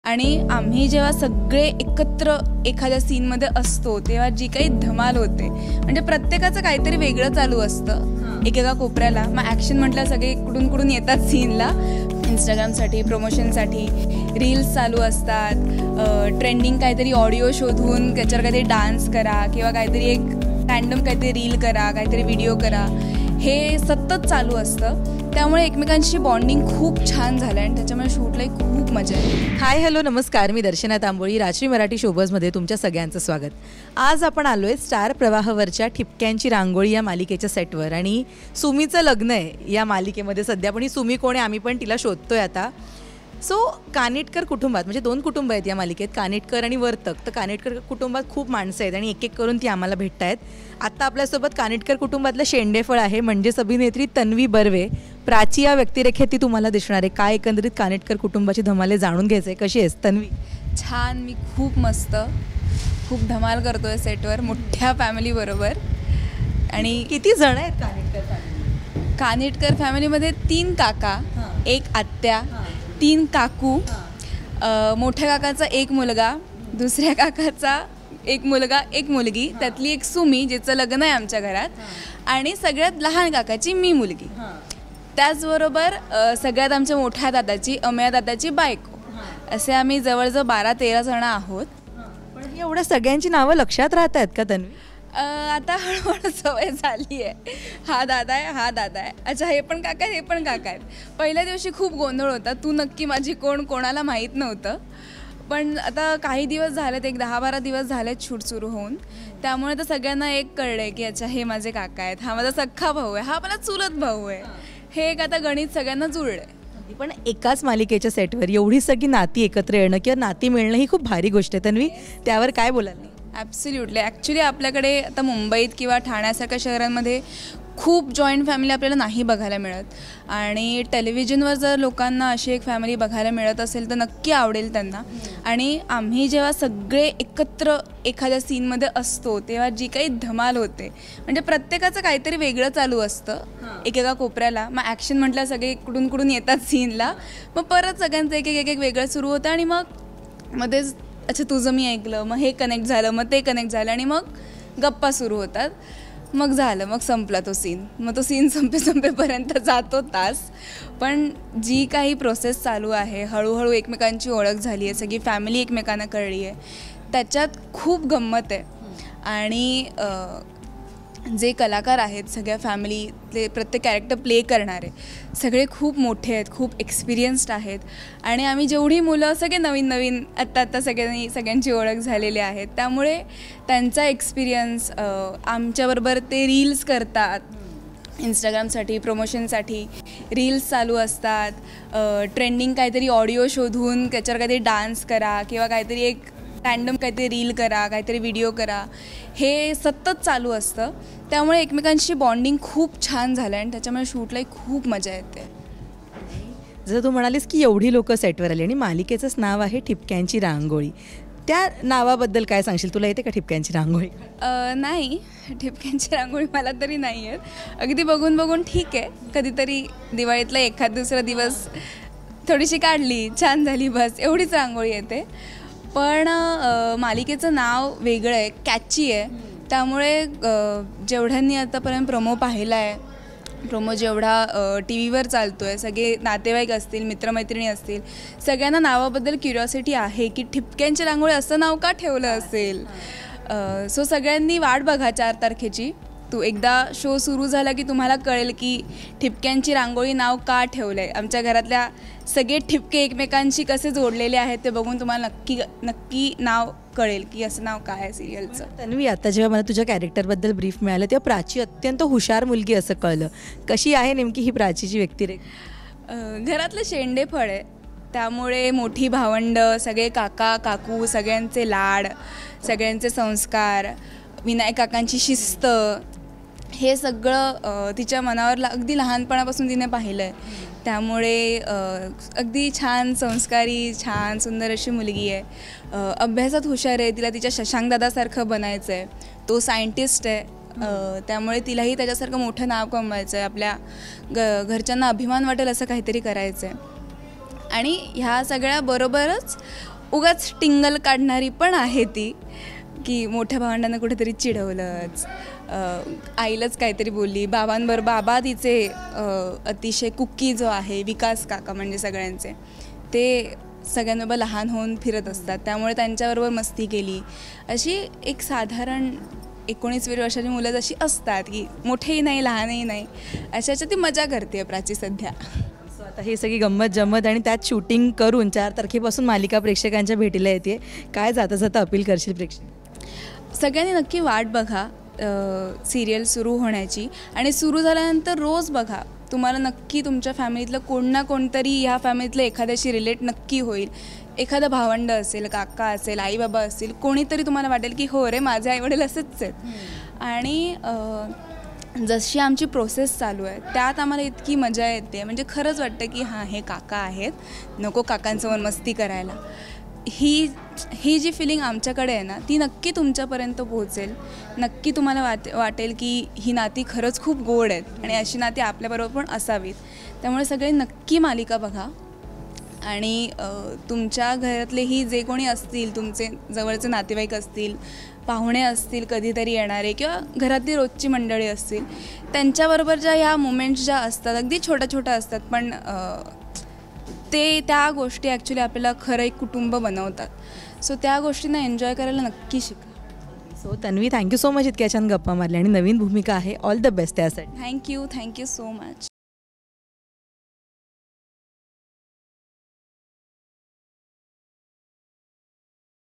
Tanvi Barve's interview | खूप लोक असल्यावर सिनमधील धमाल | Thipkyanchi Rangoli | Star Pravah
स्टार प्रवाहवर 4 ऑक्टोबरपासून ठिपक्यांची रांगोळी ही नवी मालिका सुरू होते आहे. खूप कलाकार सेटवर असतात तेव्हा सिनमध्ये कशी तारांबळ उडते ते तन्वीने सांगितलं.